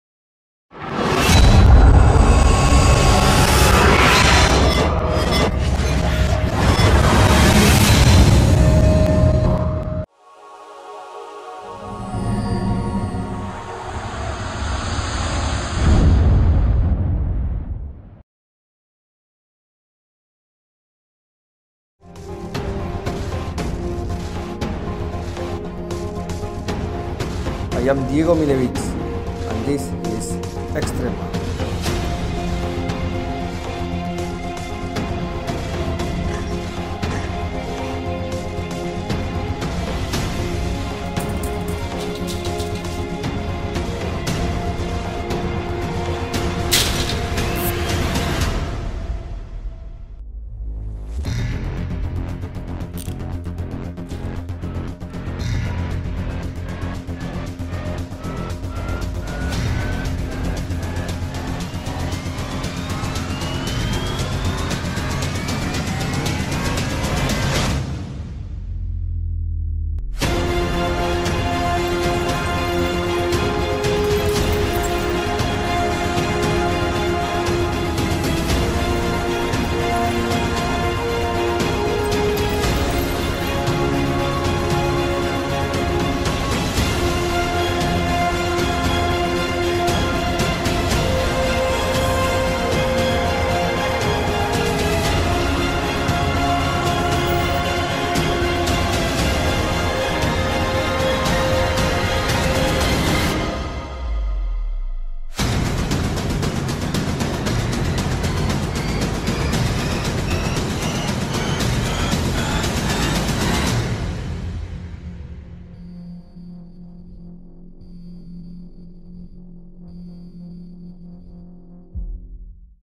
EXTREMA by Diego Minevitz & Magiclsim.
Magiclism Store and Diego Minevitz are presenting you multiple stunts of Fakirism and Escapism!

Toured internationally, the renowned extreme magician Diego Minevitz is now sharing his materials in a DVD! All contents are explained in details and shot with multiple camera angles where appropriate.

1. Fakirism
Imagine yourself walking on real pieces of glass onstage and this is only the beginning! You will be able to walk, lie down and lay your face against the glass! Diego will share his personal handling of glass. In particular, the type of glass should be used, quantity, cleaning-up and preparation. Diego's handling makes this stunt incredibly practical to perform.

2. Escapism
This is a practice of escaping from a physical enclosure of any type of trap. Diego will cover escaping and real elements releasing methods without any trickery! Including real handcuffs, tie wraps, Houdini Handcuffs and Siberian Chains. These routines are extremely practical, accommodating all types of venues ranging from close-up to stage performances. Diego will share many valuable tips and bonus performance footages of Diego's impossible escapes are also included!

This DVD is great for shows where you want to astonish your audiences, making them to believe that you possess super-human capabilities!